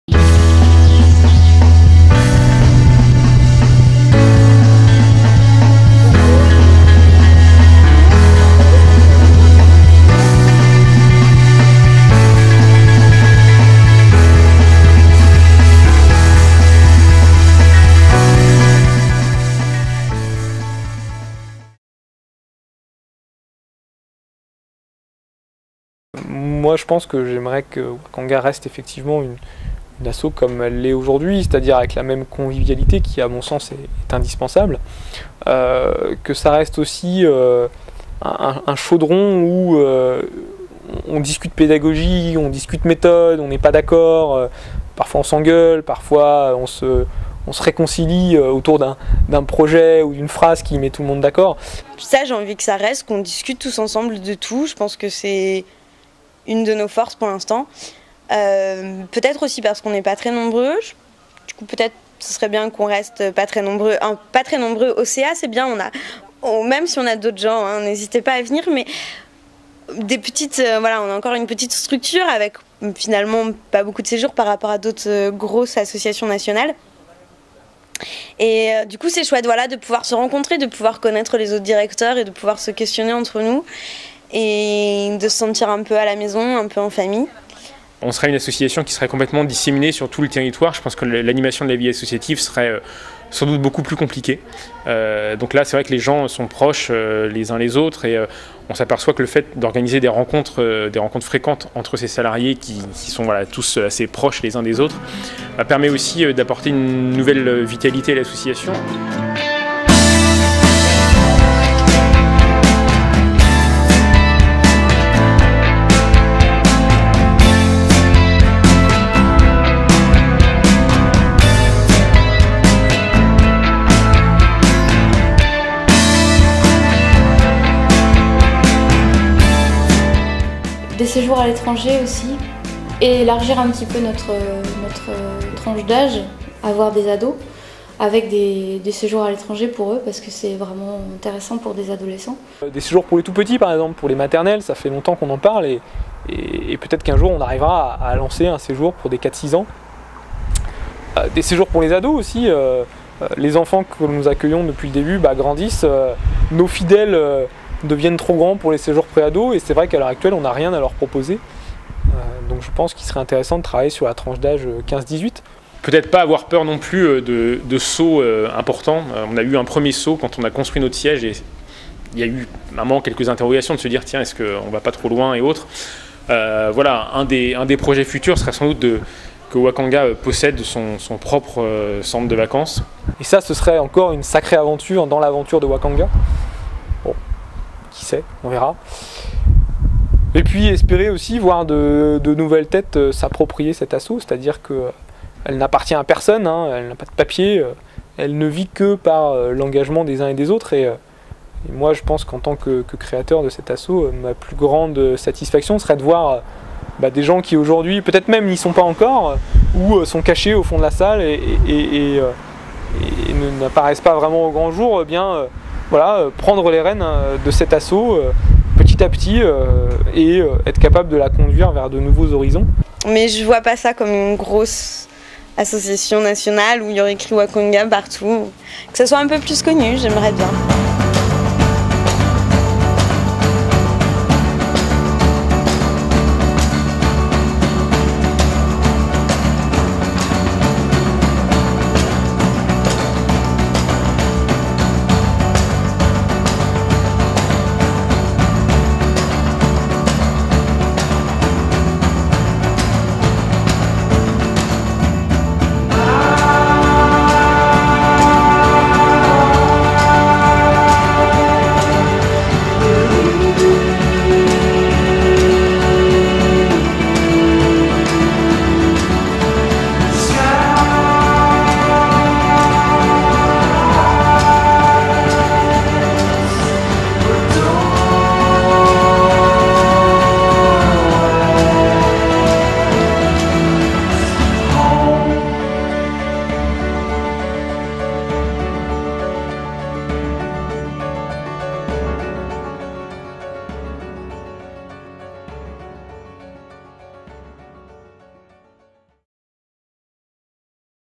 La rétro Wakanga - l'avenir de l'association
Dernier volet de notre série consacrée  l'histoire de Wakanga. Dans cet épisode, les fondateurs de l'association imaginent l'avenir de l'association à plus ou moins long terme. Il est permis de rêver un peu !